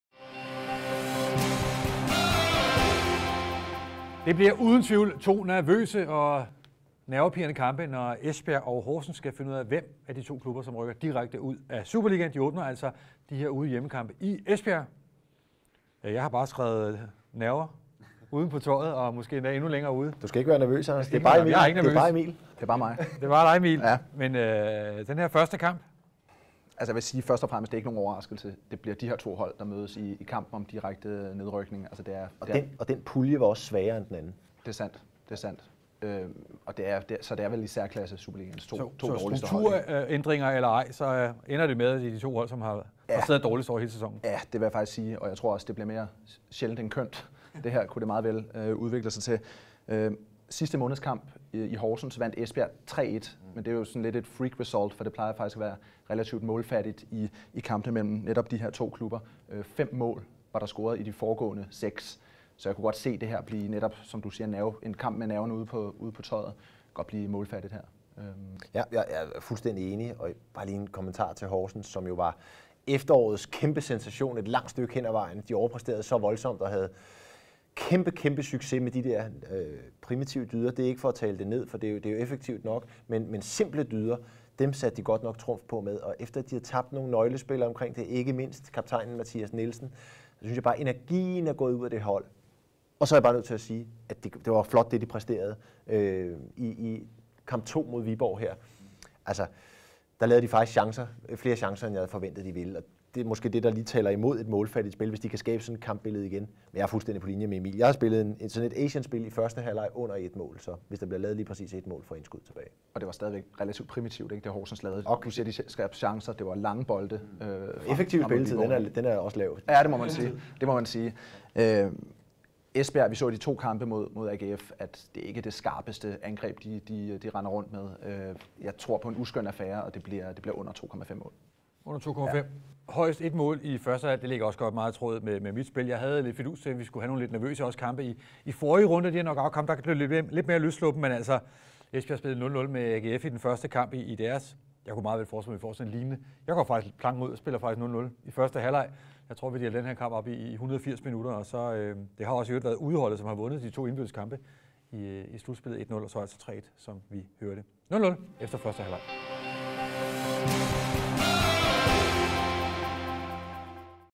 En kamp for livet!
Det er sidste chance, hvis livet i superligaen skal reddes, men for Esbjerg kan der blive tale om en langt vigtigere overlevelse. Med sidste sæsons underskud på 37 mio. kan en nedrykning blive fatal. Spillerne har altså også deres eget liv på spil. Nervøsiteten breder sig, men eksperterne ryster ikke på hænderne, når de giver deres spiltips lige her.